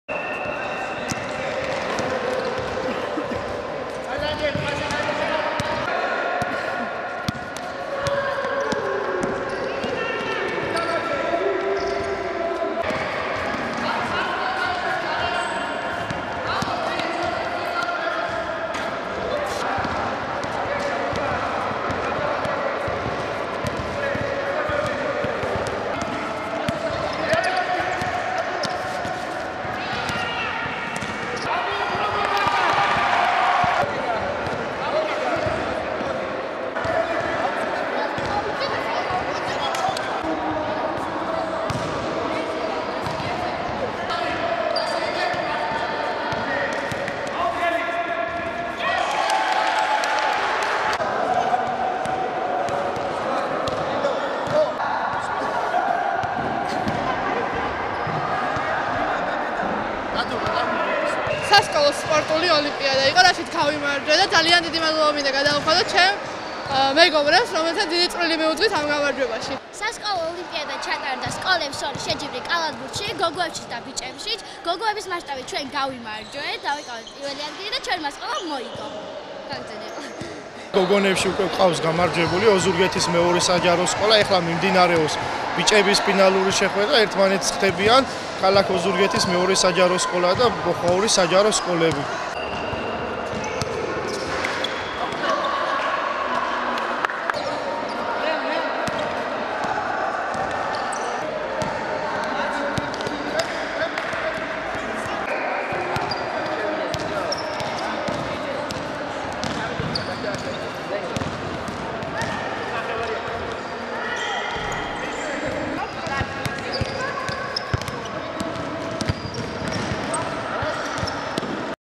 სასკოლო ოლიმპიადა კალათბურთში
სასკოლო ოლიმპიადა კალათბურთში დასრულდა.
გოგონებს შორის ფინალური მატჩი მეორე საჯარო სკოლასა და სოფელ შრომას საჯარო სკოლის გუნდებს შორის გაიმართა, რომელიც მეორე საჯარო სკოლის გამარჯვებით დასრულდა. ბიჭებს შორის ფინალში ერთმანეთს მეორე საჯარო სკოლისა და სოფელ ბოხვაურის საჯარო სკოლის გუნდები შეხვდნენ. შეხვედრა ამ შემთხვევაშიც მეორე საჯარო სკოლამ მოიგო.